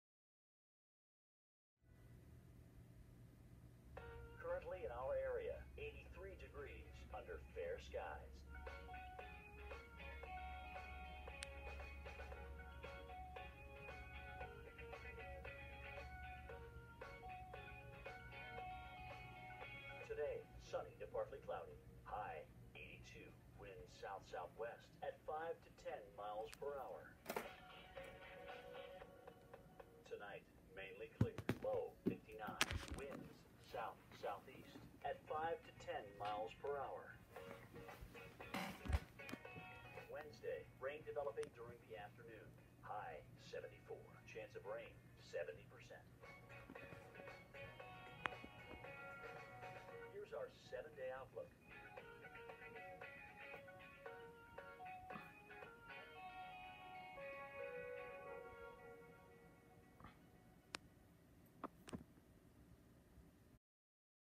(READ DESCRIPTION) TWC IntelliStar 2 XD - Valparaiso, IN June 11, 2019
I had to re-record this video due to it half the image being away from the TV. Anyway, here’s a video from nearly two years ago that I forgot to upload.